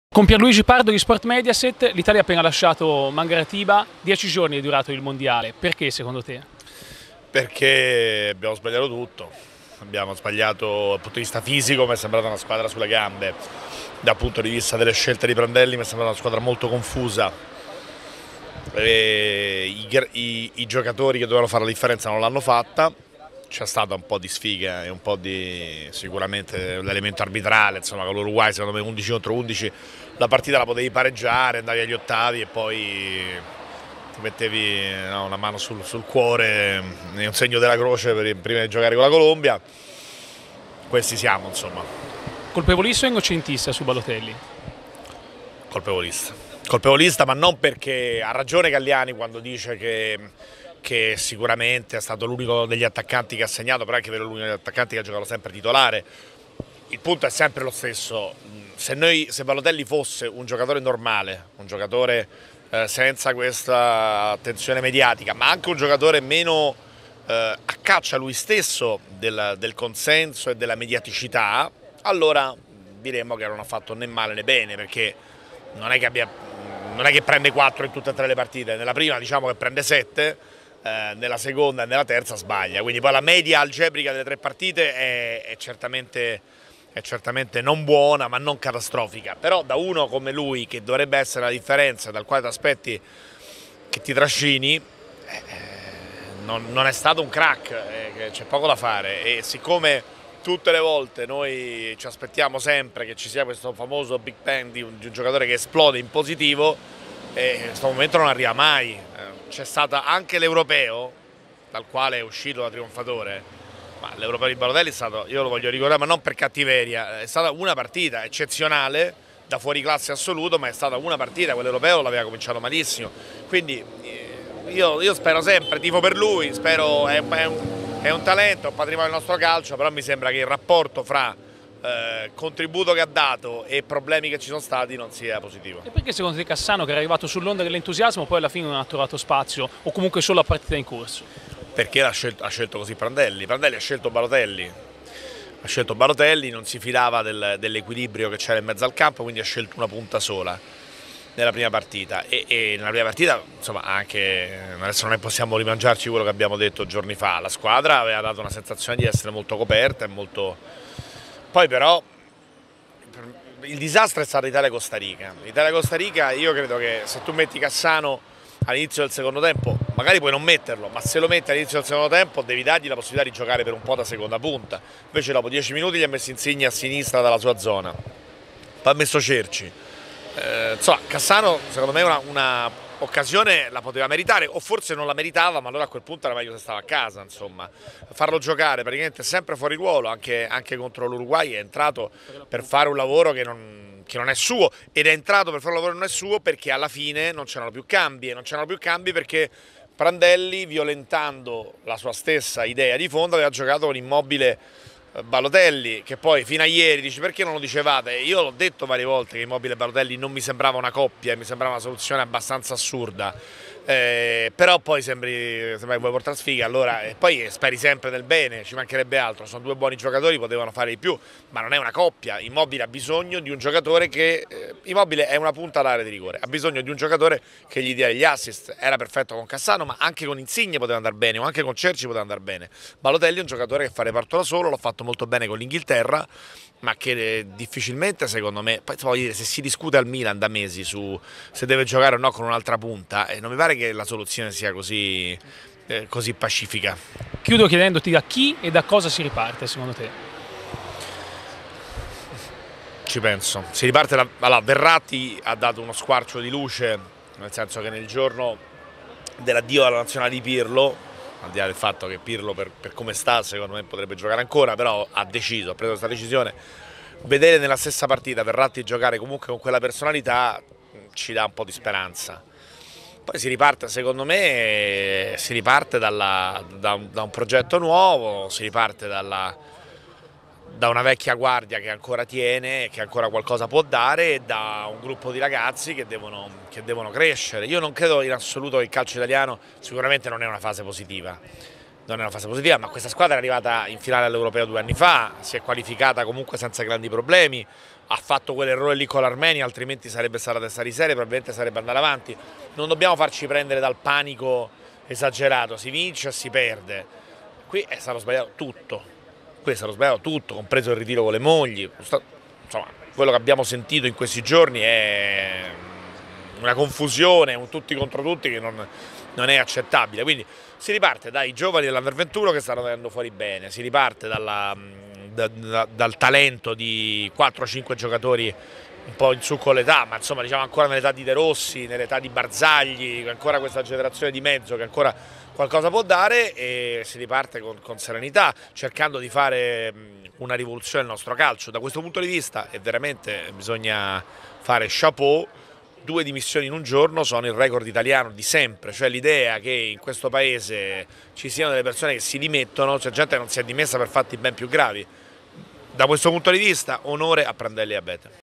Pierluigi Pardo (Sportmediaset): "Balotelli? Non positivo rapporto tra contributo dato e p
Intervista a Pierluigi Pardo (Sportmediaset): "Balotelli? Non positivo rapporto tra contributo dato e problemi creati"